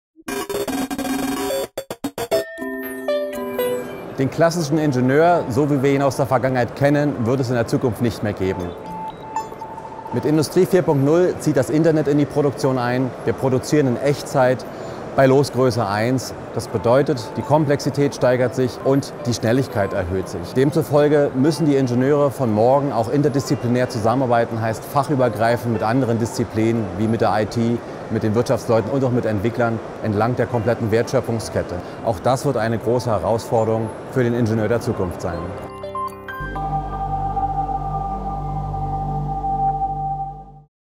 Andreas Schulz: Ingenieure von morgen müssen interdisziplinär arbeiten!
"Tomorrow's engineers have to work interdisciplinary!"
In this edition of Thoughts of the Week, tech-blogger Andreas Schulz of ingenieurversteher.de talks about Industry 4.0, the Internet of Things and the digitalisation of our society in general.
"Ingenieure von morgen müssen interdisziplinär arbeiten!"
Der Technik-Blogger Andreas Schulz von ingenieurversteher.de hat die Fokusthemen rund um Industrie 4.0, Internet der Dinge und allgemein die Digitalisierung unserer Gesellschaft.